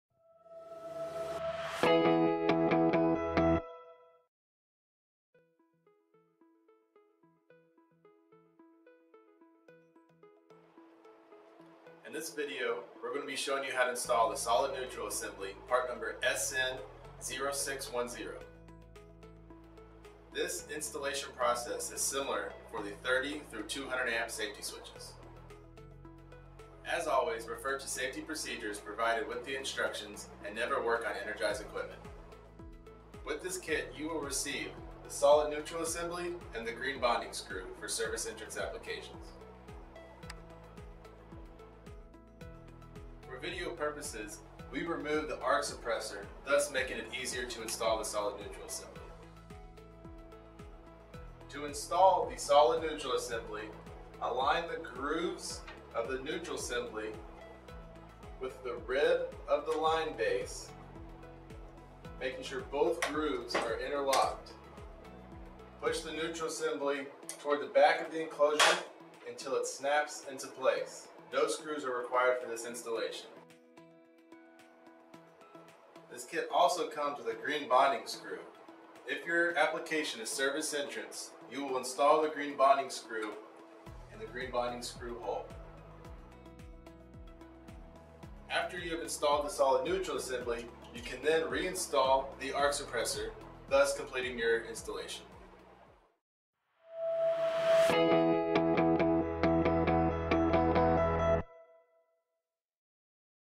Installing SN0610 Neutral Assembly into an F-Series Heavy Duty Switch
Tutorial for installing a SN0610 solid neutral assembly into a Square D F-series heavy duty safety switch with max ratings between 30A and 200A.

NOTE: This video is a supplement to the instructions included with the equipment. Please read and understand these instructions before working on any electrical equipment.